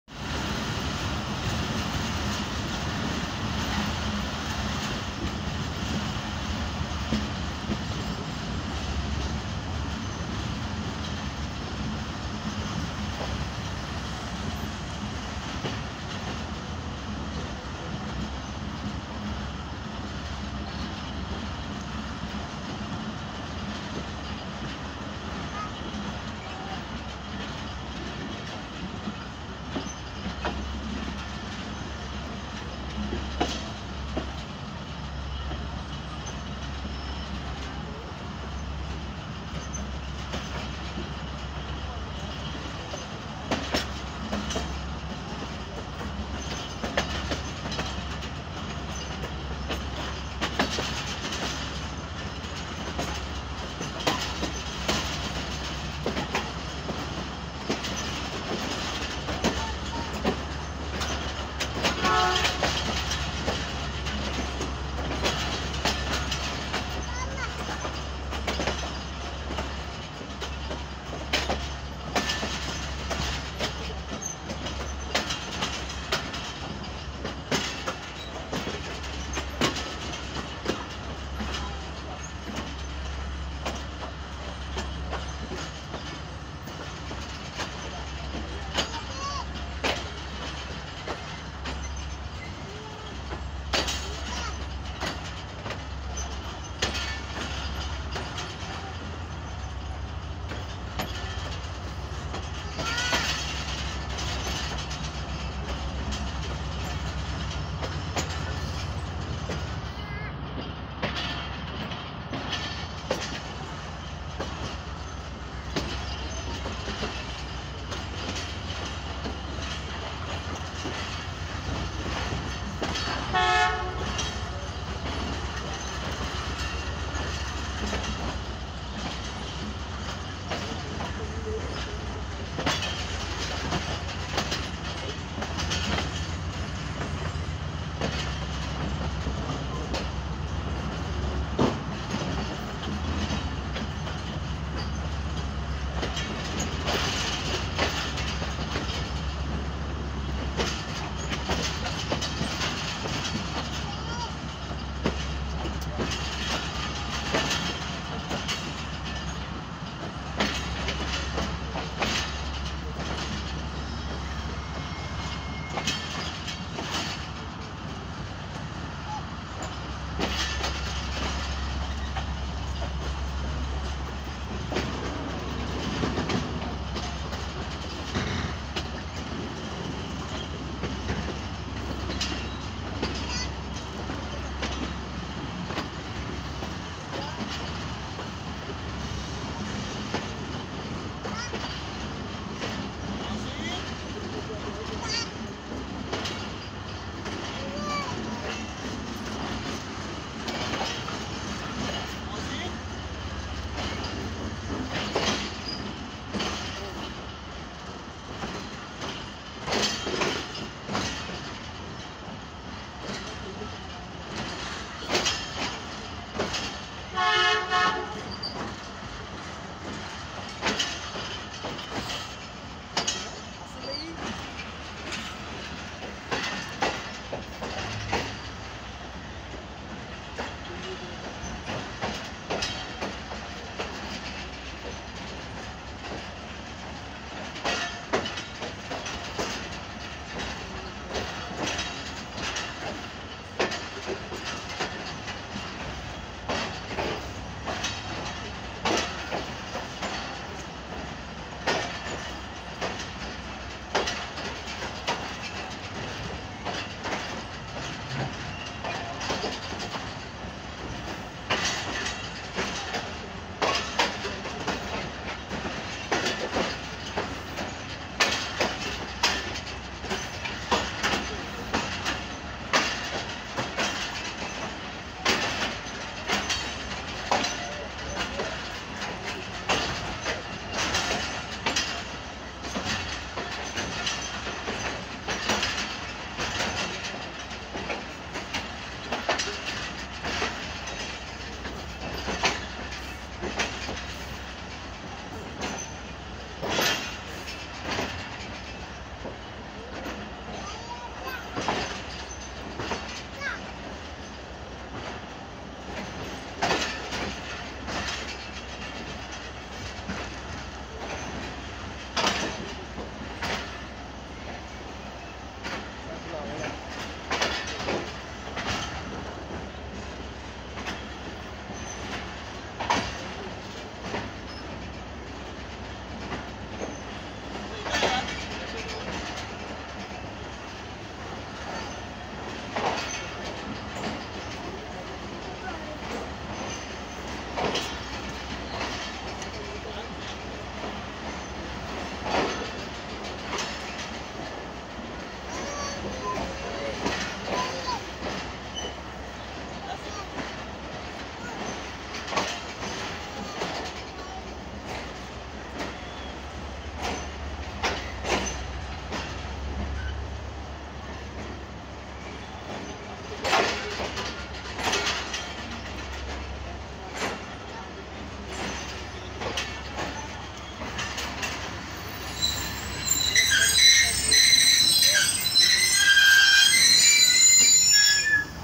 Pakistan Railways:10dn Allama Iqbal Express reaching Hyderabad Junction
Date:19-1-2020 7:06